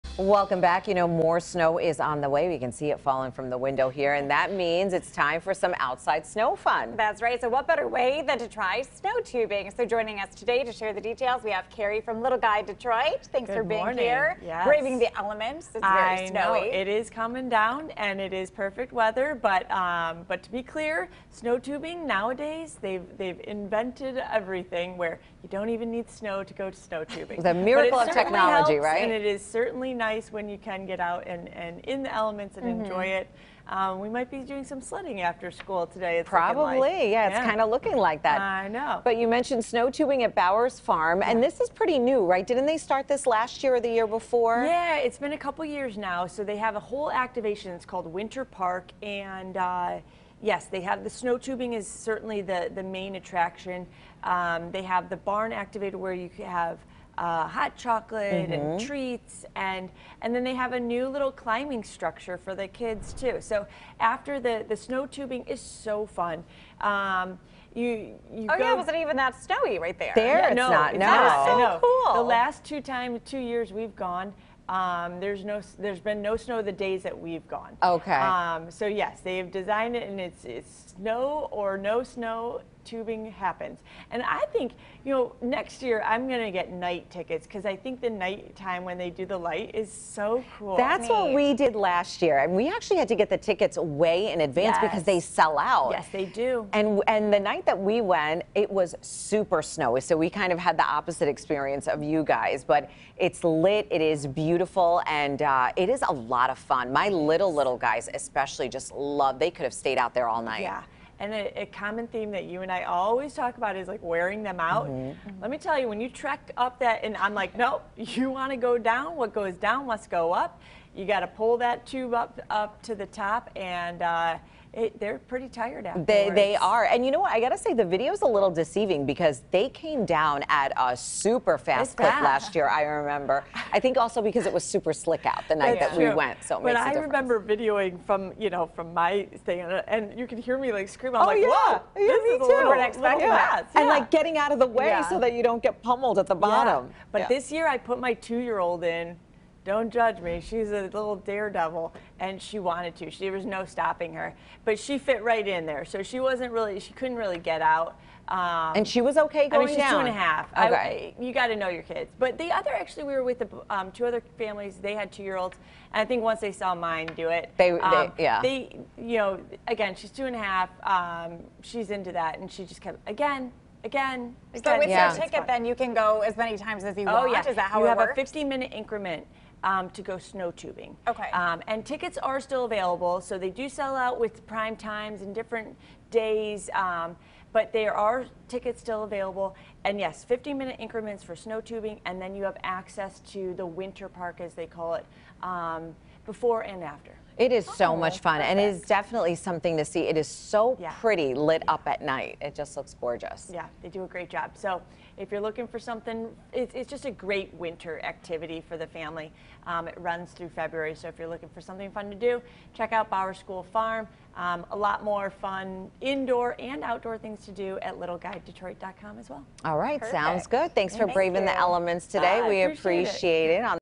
LittleGuide Detroit: Fun winter activities at Bowers School Farm in Bloomfield Hills
Kerry Doman, founder of LittleGuide Detroit, joined CBS News Detroit with a look at fun winter activities at Bowers School Farm in Bloomfield Hills.